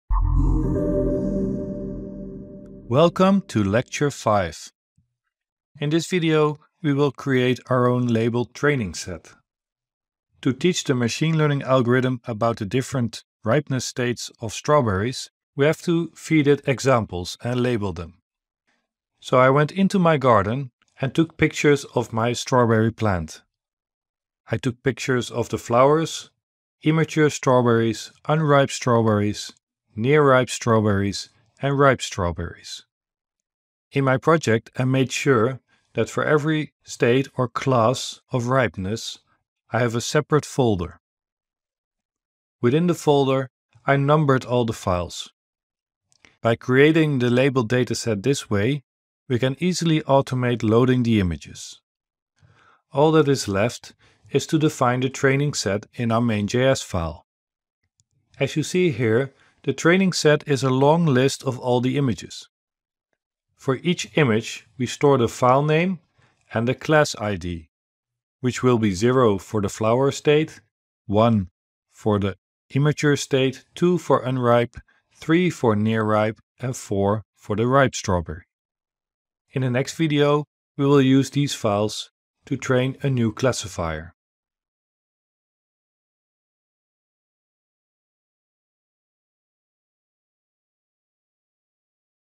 ML - 04 Training set
In this video, we will create our own labeled training set. .

Fontys for society
This video is created by Fontys University of Applied Science - school of ICT (Eindhoven, the Netherlands) as part of the EMR Digital Twin Academy  Strawberry Fields case study.